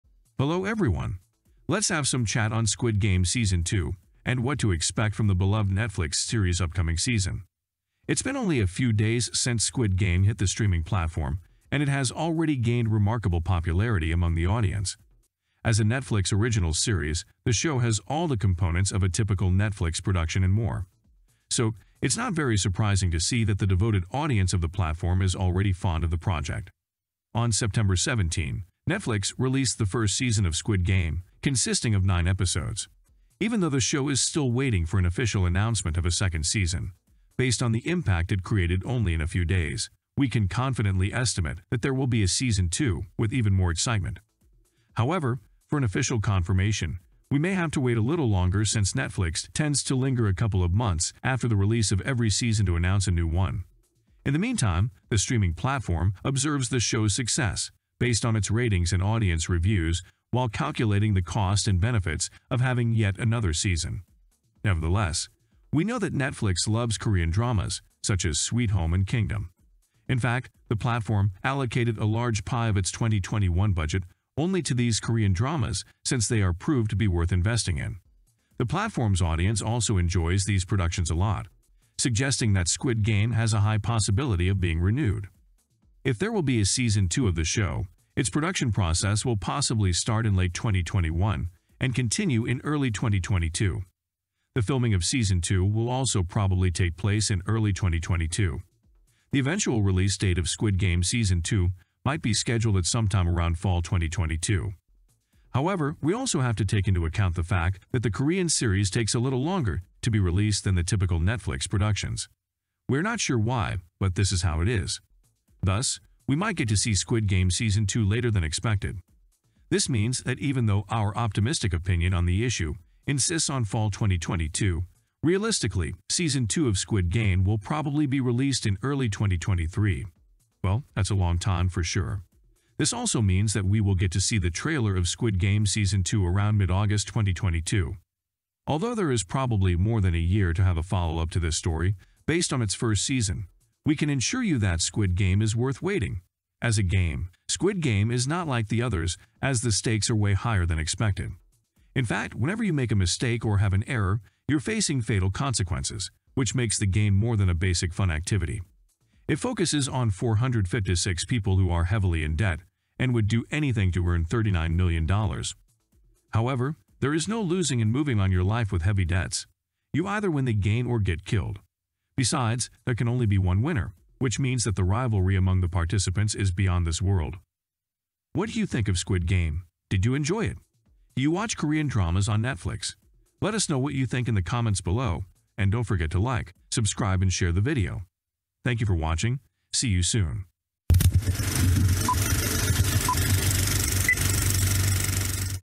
Squid Game Season 2 Trailer, Release Date (PREDICTIONS)
Today we'll talk about Squid Game Season 2 Release Date on Netflix and Squid Game Season 2 Trailer. Let’s have some chat on Squid Game Season 2 and what to expect from the beloved Netflix series’ upcoming season. If there will be a Season 2 of the show, its production process will possibly start in late 2021 and continue in early 2022.